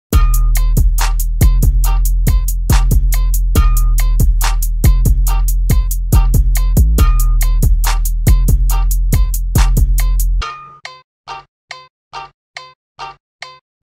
Ukelelelele
Wacky ukelele animation test. Can it groove? My, yes it can.